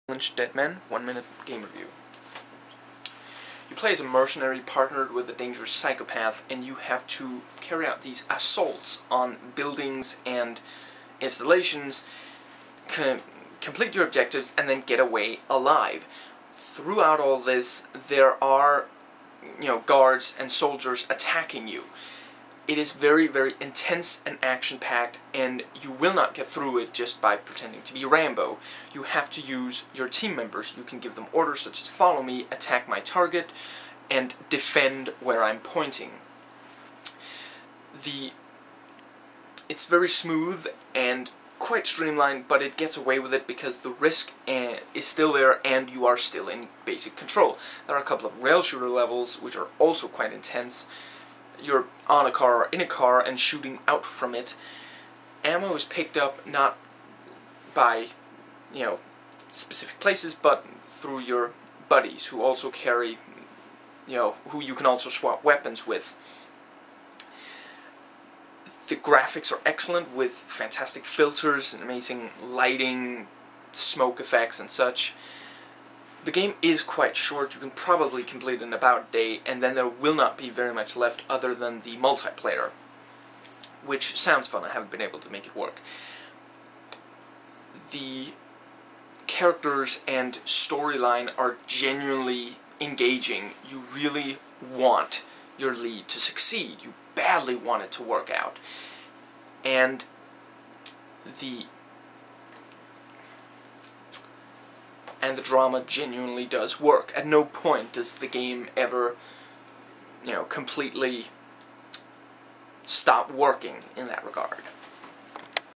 Kane & Lynch: Dead Men game 1min review
I recorded this today, after having completed the game late last night. I hope you enjoy it, and that I can help spark debate about the game.